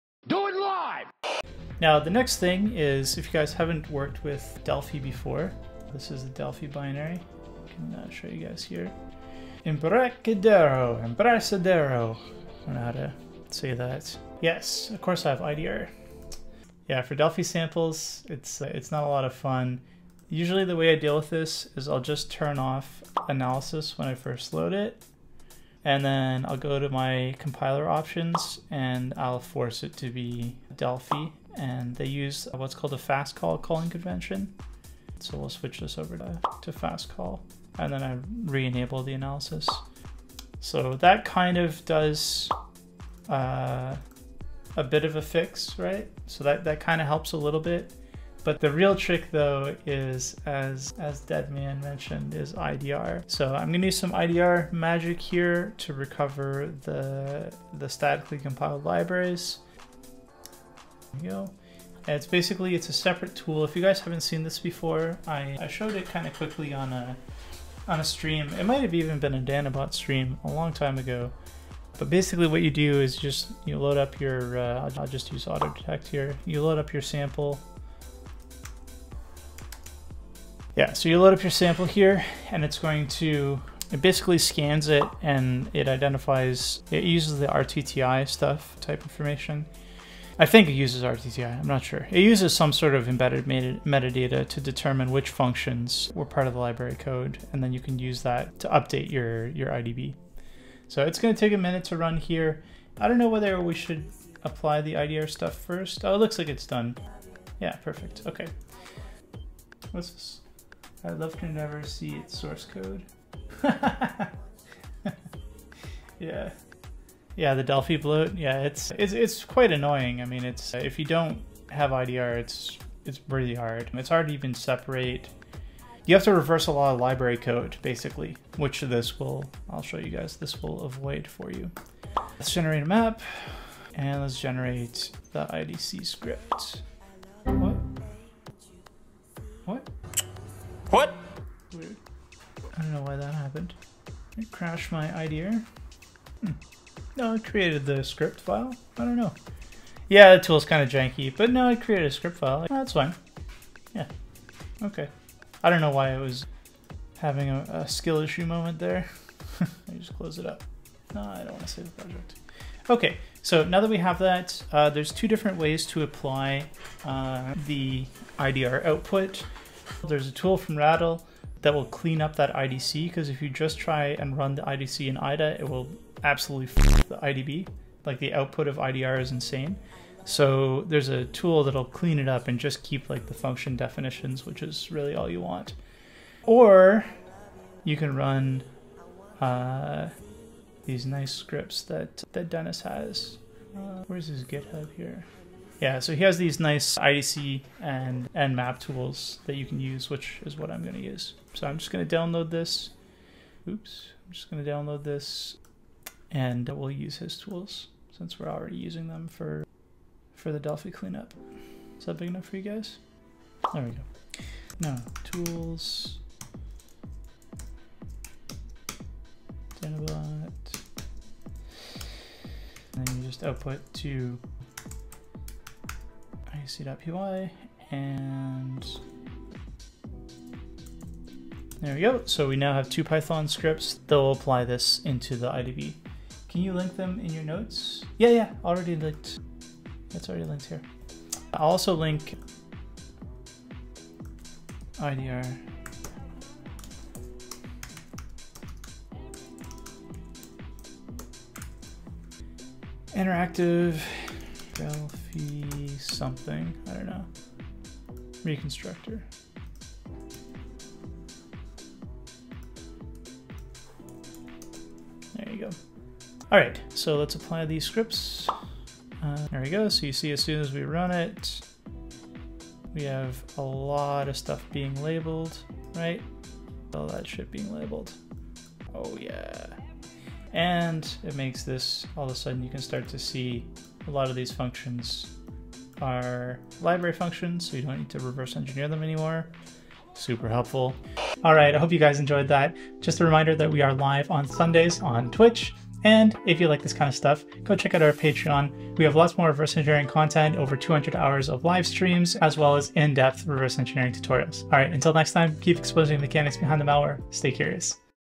Tips For Analyzing Delphi Binaries in IDA (Danabot)
Reverse Engineering Delphi is a nightmare ... or it can be if you don't have the right setup! In this clip we cover some easy tips that can help make some of the analysis a bit easier.